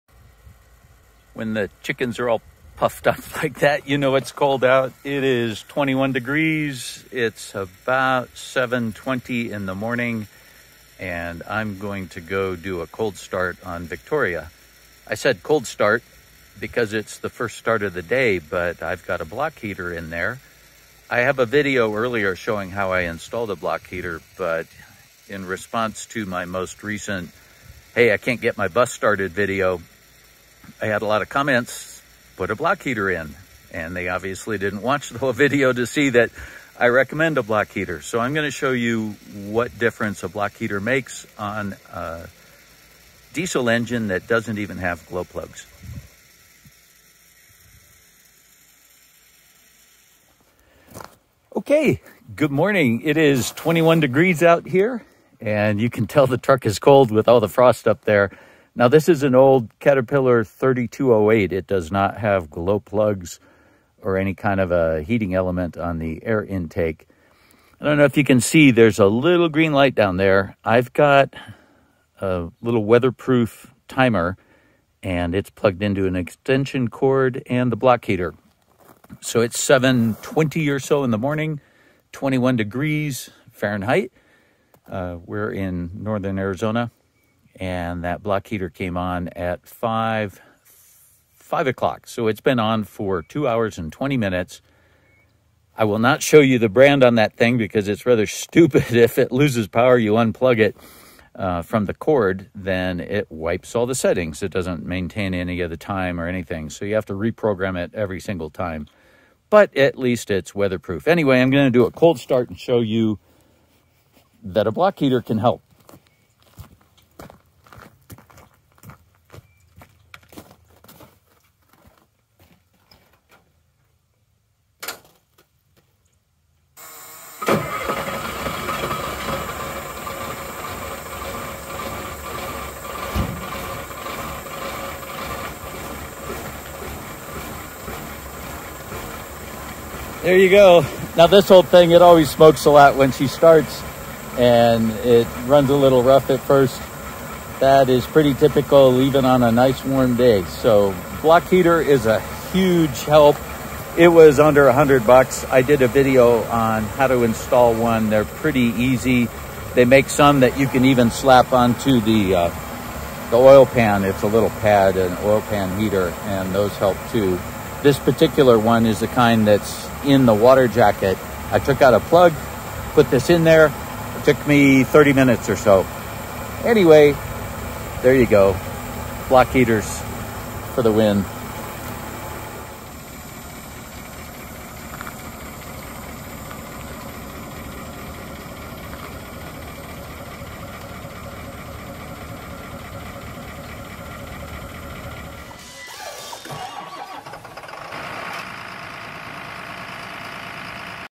Does a block heater help? Yes! (Cat cold start)
This is a short video showing a cold start on this 21F morning.  The engine is a Caterpillar 3208, which has no glow plugs or intake air heater...so the block heater is a must-have item in cold weather.  The heater's been on for 2 hours and 20 minutes.

The truck is a 1989 Ford C8000, which started life as a road painting truck for the Iowa DOT.  Since it only painted roads in the summer, it had no block heater.  I added one and you can find that video here: